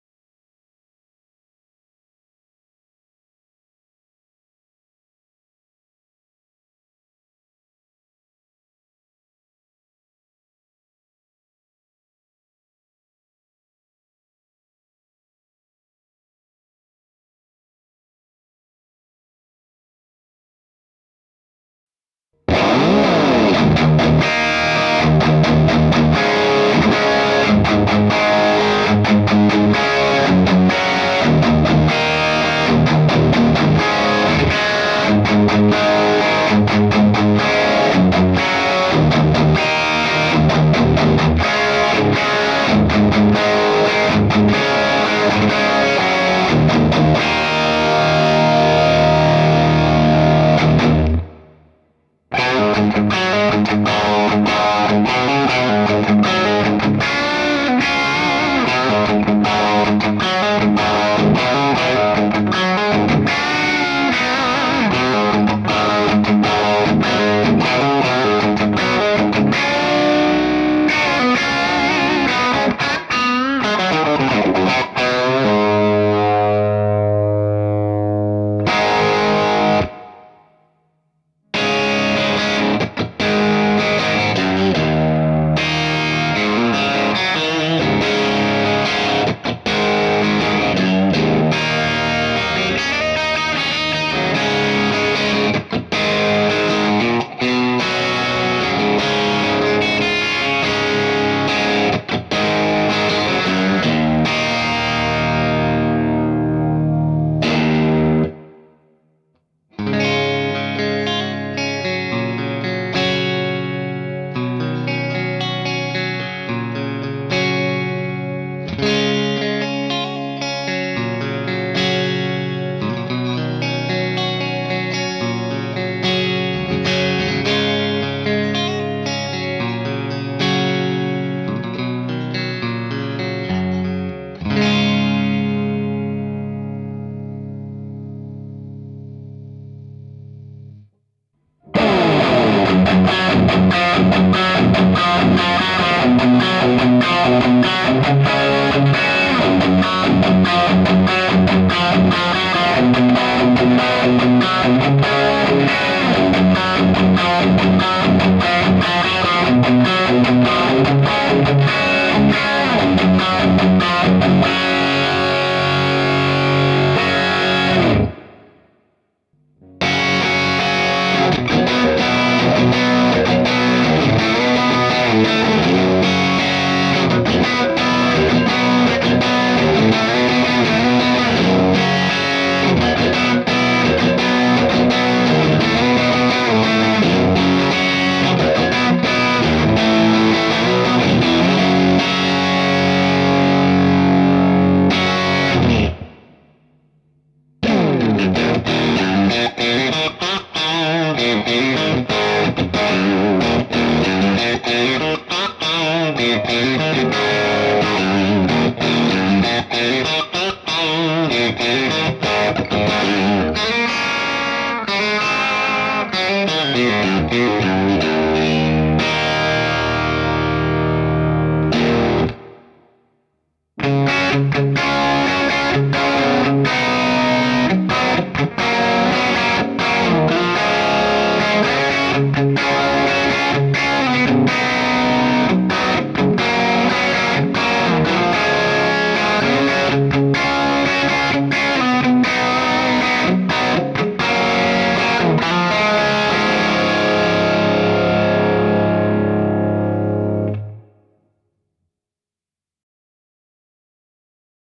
Top Jimi "Tangerine 30" Pack Playthrough (Orange AD30)
Playthrough of some of the Kemper amp profiles in the Top Jimi "Tangerine 30" profile pack.